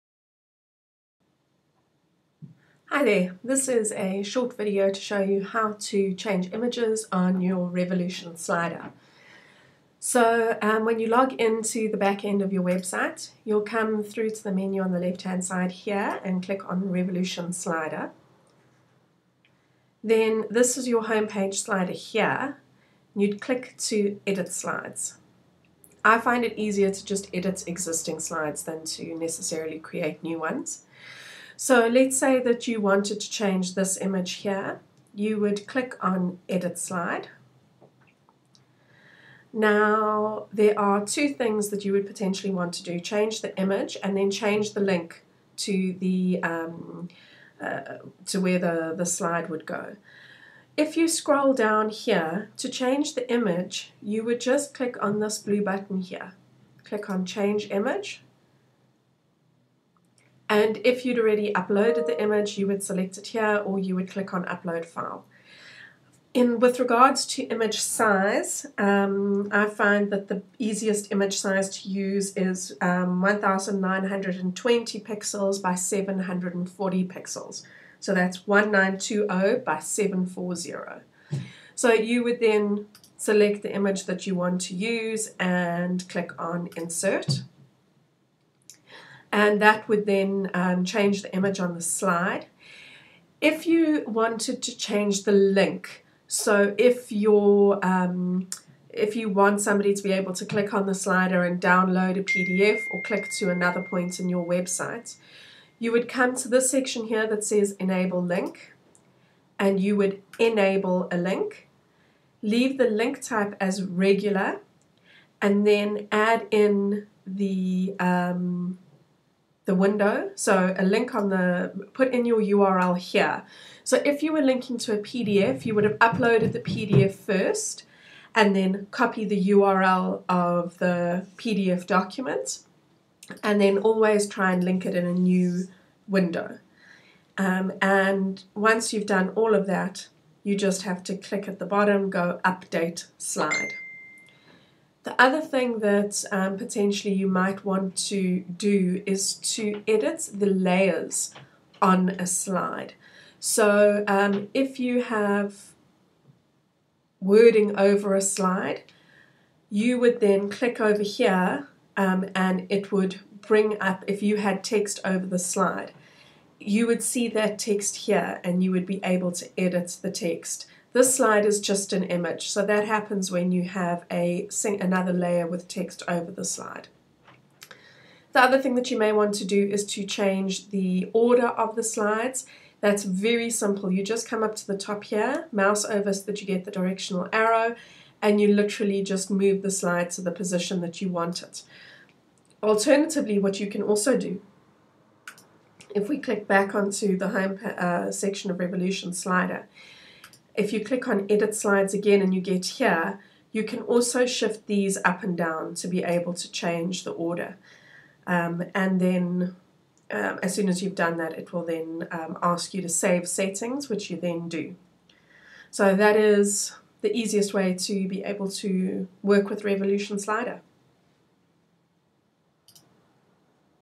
How to change images on Revolution Slider
I find that the best size for the image is 1920x740. Try to compress the image size as much as possible, large image files will slow down the load time of the site.